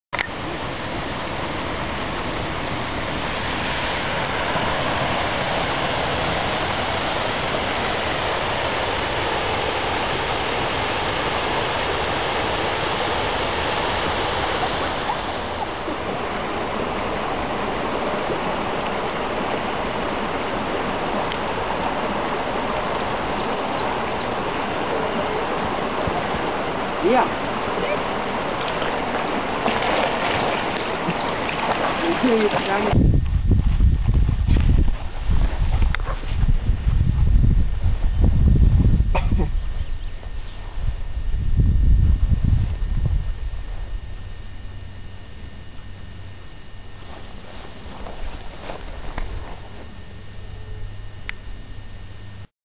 Imroy in les Hautes Fagnes
the Welsh springer spaniel Imroy is enjoying the beautiful nature of les Hautes Fagnes in Belgium.